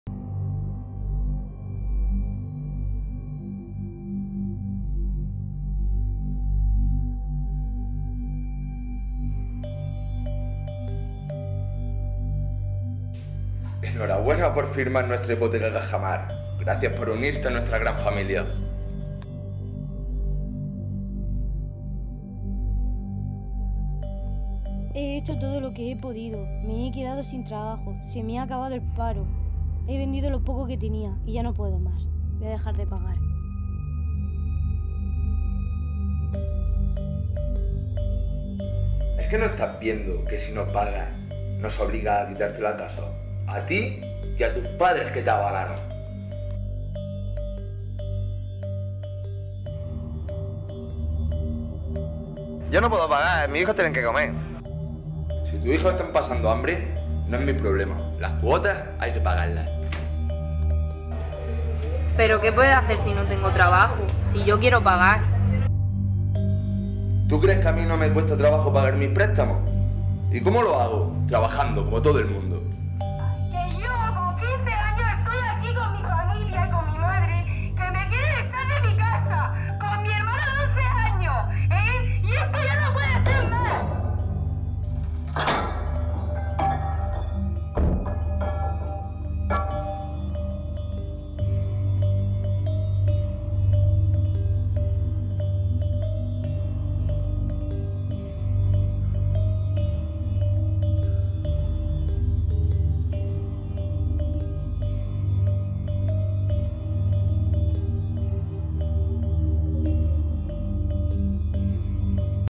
Cajamar colma el vaso
Campaña de las PAHs Murcianas para conseguir soluciones de las afectadas por Cajamar.